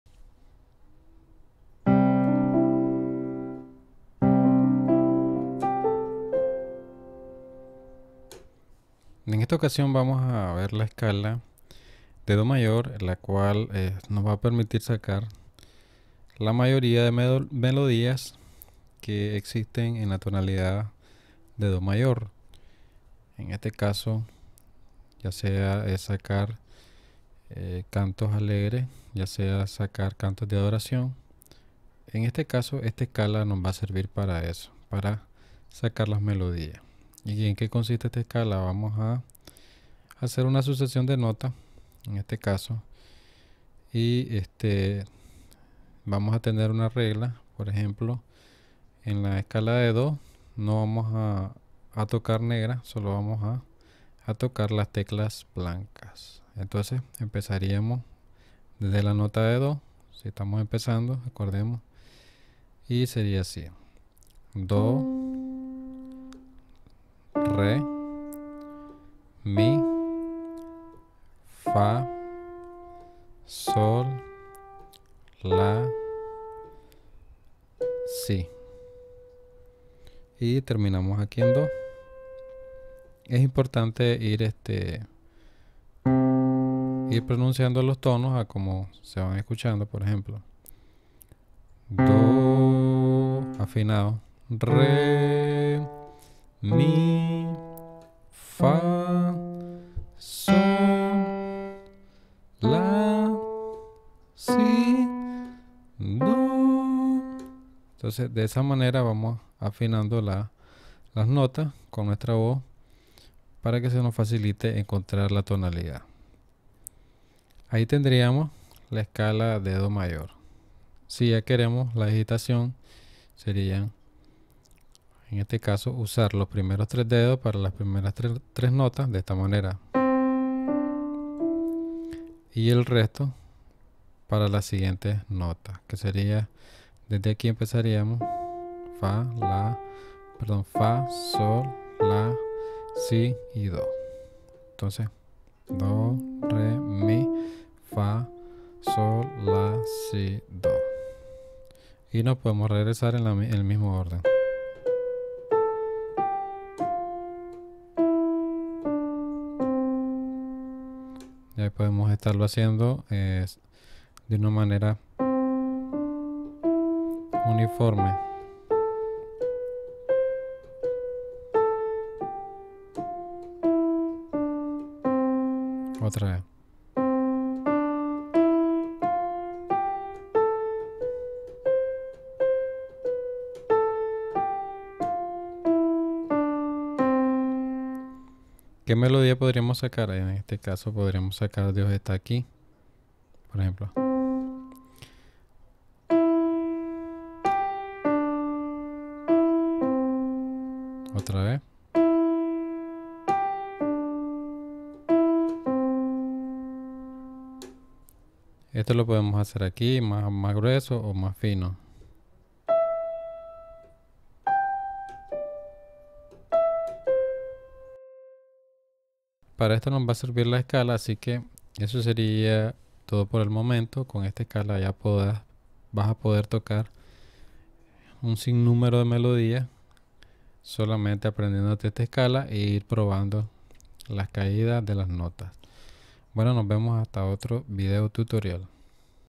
Escalas / Aprende a Tocar canciones con esta Escala de ( C M ) | Piano Básico 3
Como se realiza la escala mayor de Do Mayor con su digitación para sacar muchísimas canciones fácilmente.